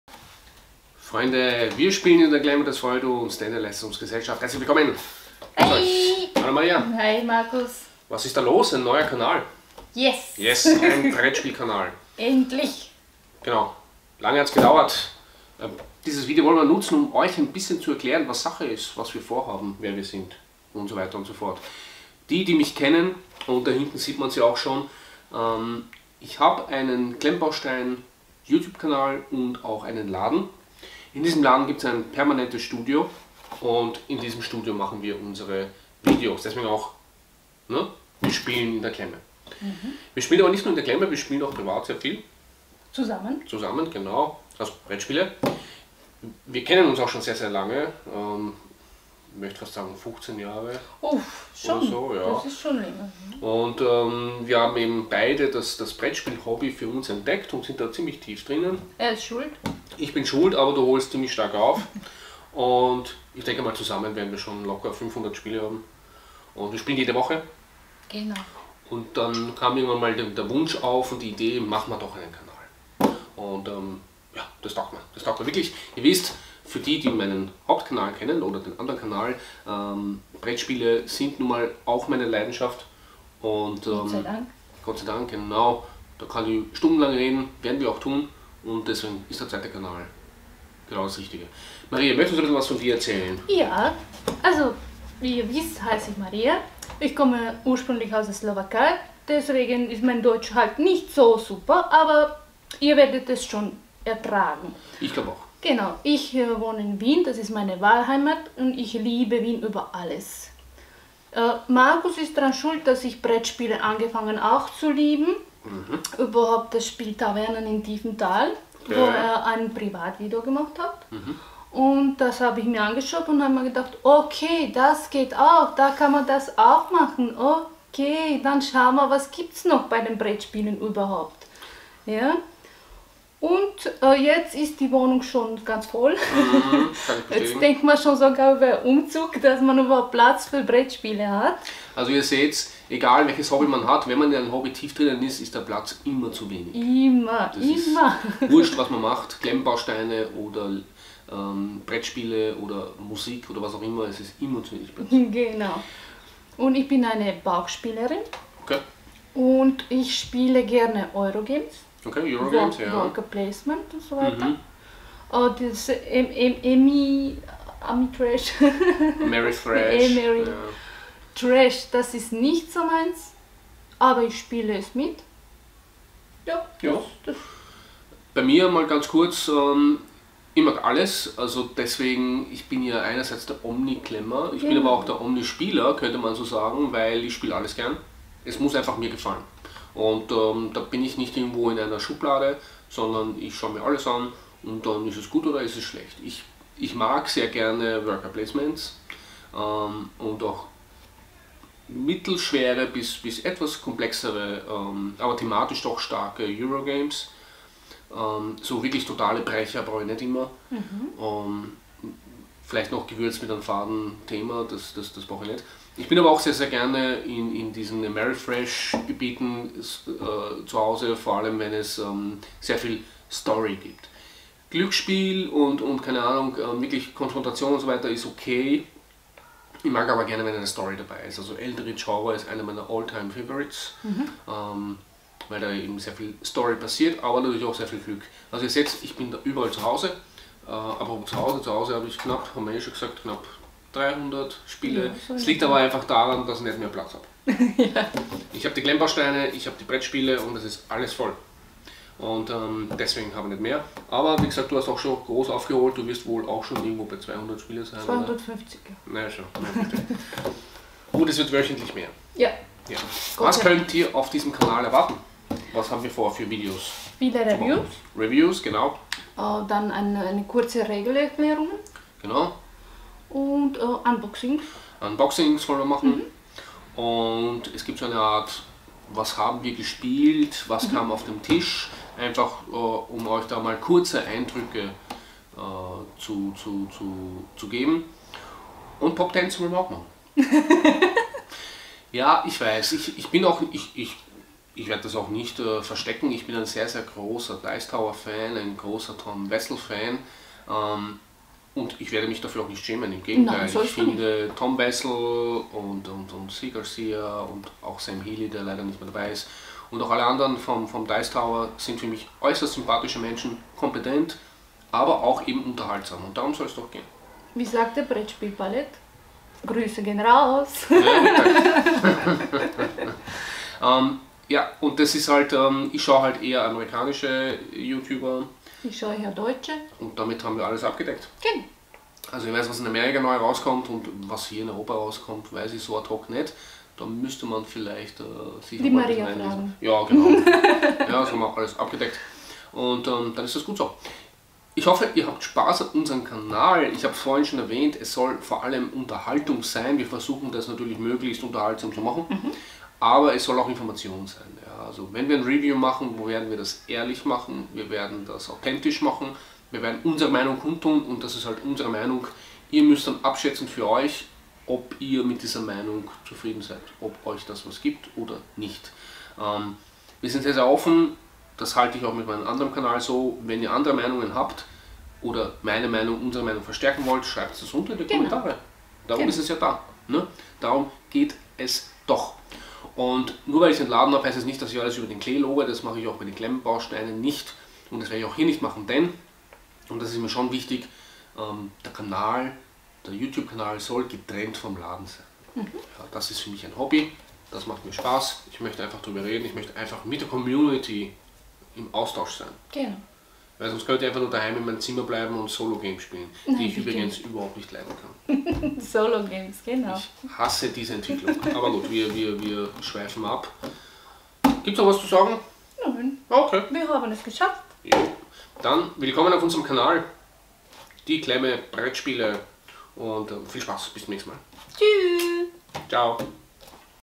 Unser erstes Video: Wir stellen uns vor!
Wir - Maria und Markus - sind 2 begeisterte BrettspielerInnen, und möchten einfach mehr aus unserem Hobby rausholen. Wir lieben es  über Spiele zu reden. Und wir wollen Gleichgesinnten und Interessierten informative und unterhaltsame Videos bieten.

Ich - Markus -  habe einen Klemmbausteinladen in Wien - das erklärt ein wenig den Titel. Und auch schon einen Yotube-Kanal - schaut auch da mal vorbei:

Die Klemme - Brettspiele
Wallensteinstrasse 2
1200 Wien